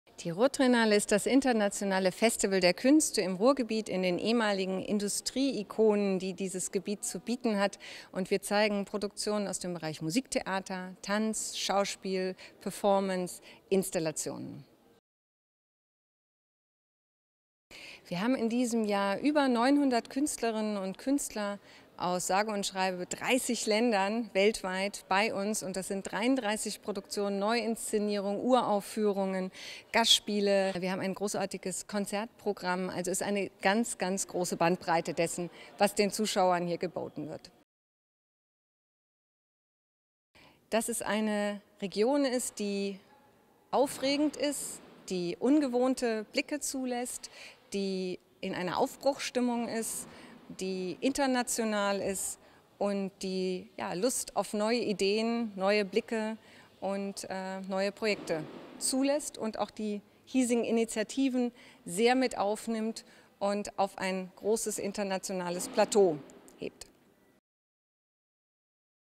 Die Ruhrtriennale 2018 - Vera Battis-Reese im Interview
Heute Abend feiert die Ruhrtriennale Premiere. Worauf der Zuschauer gespannt sein darf, erzählt uns Vera Battis-Reese, Geschäftsführerin der Kultur Ruhr GmbH. Das internationale Festival der Künste trägt auch zum neuen Bild der Metropole Ruhr bei. Welches das ist, verrät sie uns ebenfalls.